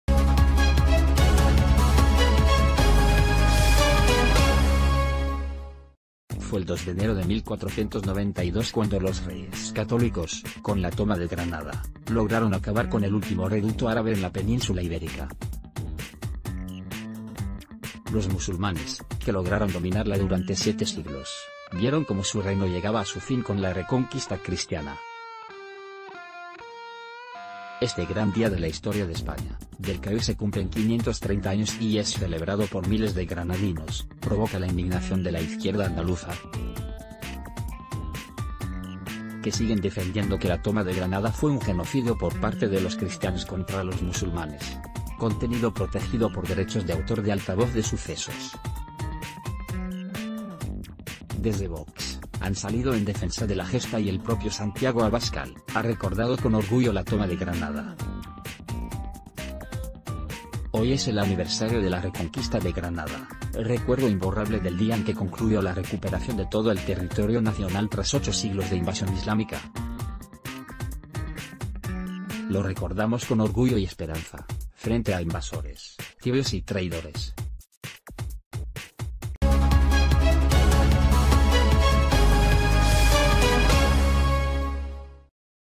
VOX celebra la Toma de Granada que «supuso el fin de la invasión islámica»: «Lo recordamos con...
VOX celebra la Toma de Granada que «supuso el fin de la invasión islámica»: «Lo recordamos con orgullo y esperanza»